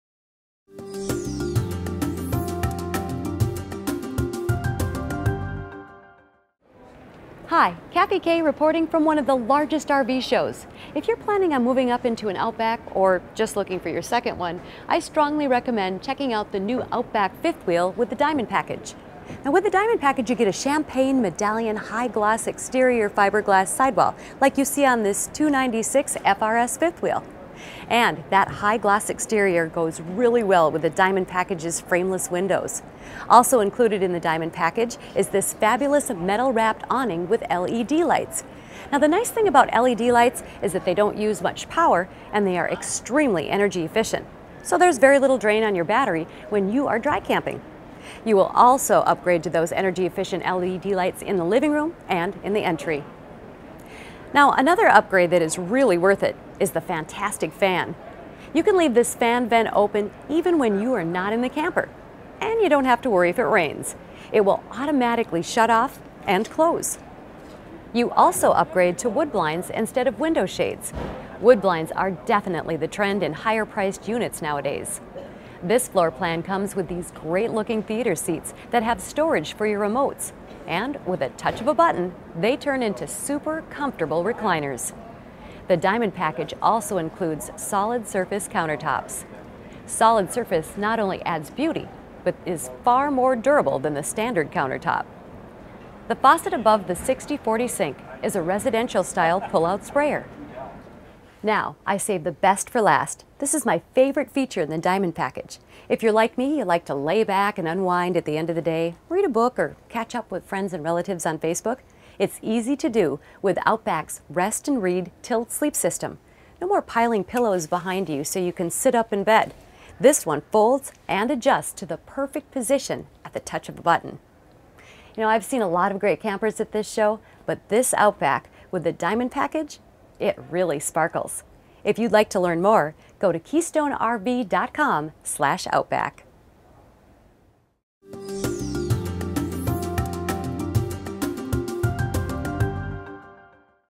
Outback Camper Diamond Package Feature Tour
Outback is already one of the best selling campers of all time with more than 50,000 sold.  This new Diamond Package pushes it over the top with features like high-gloss gel-coat exterior, frameless windows, LED lighted power awning with metal-wrapped cover, solid surface kitchen countertop, LED interior lighting, upgraded furniture, and the exclusive Rest-n-Read bed.